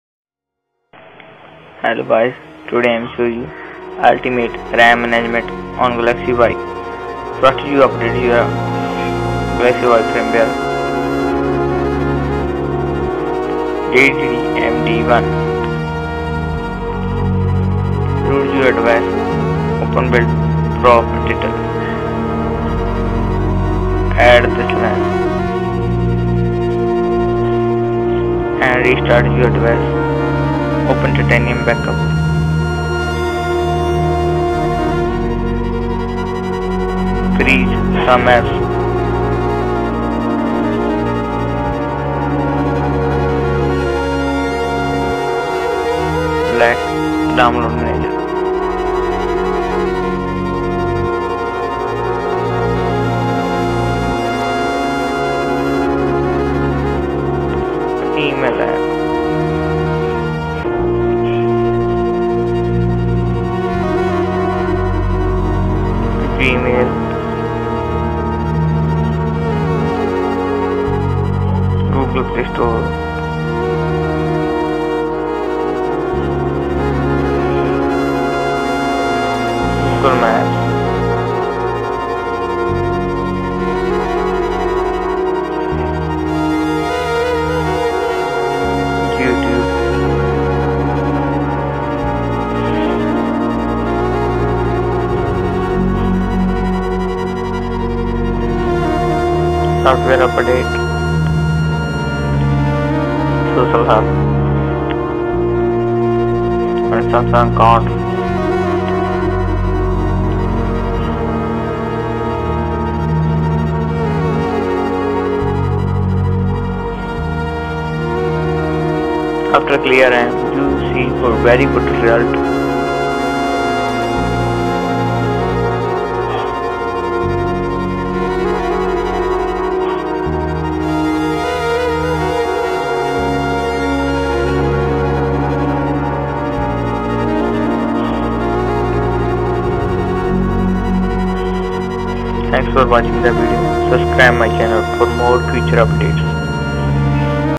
How to get more free RAM on galaxy Y (86-289 mb)
Frist you update your galaxy y with latest frimware DDMD1.This is stable release by samsung.
Edit build.prop and add line
ro.HOME_APP_ADJ=1

Use any bulid.prop editor like rom toolbox
Why You Apply This On your Galaxy y
1. More free ram more smooth exprince you use galaxy y .Galaxy run very fast and never slow down again.
2. When media scanning is runnig your device never slow again.
3. Run Heavy Apps without any problem Like Screen cast ,Mozila Firefox Browser Very Smoothly.
4.Fast Internet Speed.Because system application always run in the background. This Apps always use More Ram And Always Use internet and Slow Down your Internet Speed. (I think 2g Users experience of this always )
:( Very Very Sorry For Bad English In the Video :(
When I clear the ram, Ram Goes minimum to 86 mb